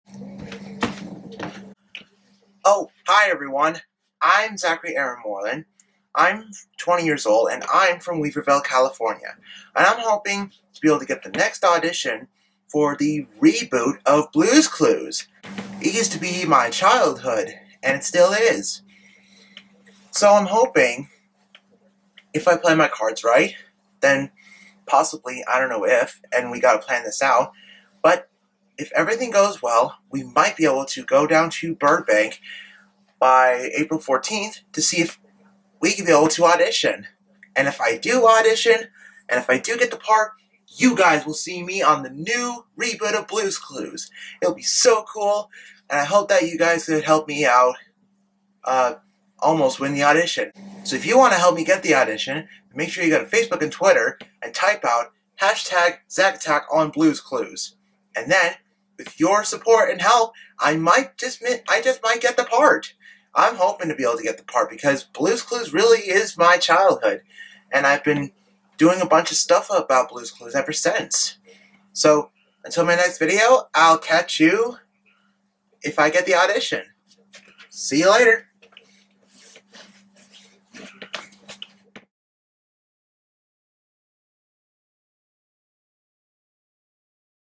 Blues Clues Reboot Audition Video
I hope I get the part of the new blues clues host for the reboot show!